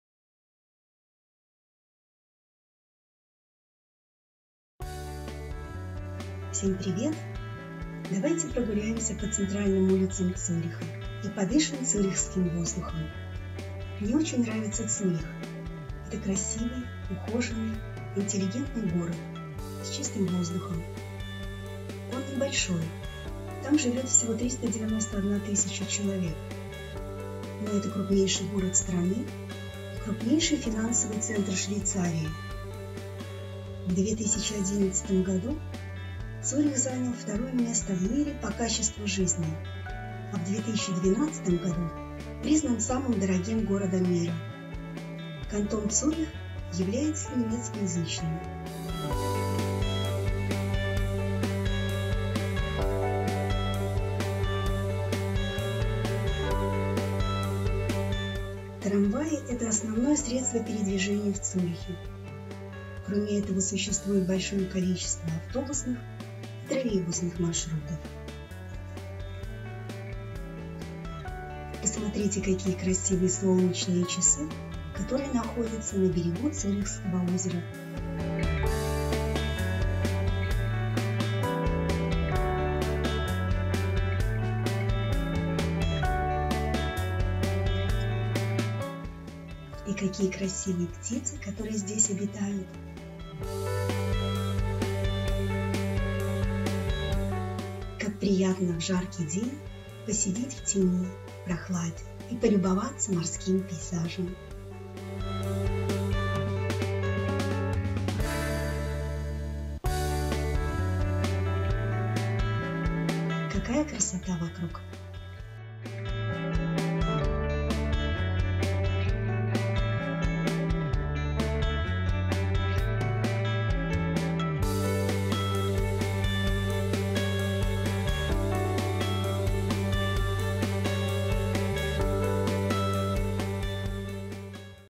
Лето в Цюрихе. Цюрихское озеро. Солнечные часы. Цюрих. Швейцария. ВЛОГ. VLOG Zürich
Всем привет!

Предлагаю прогуляться по центральным улицам Цюриха, посидеть в тени у Цюрихского озера. Швейцария.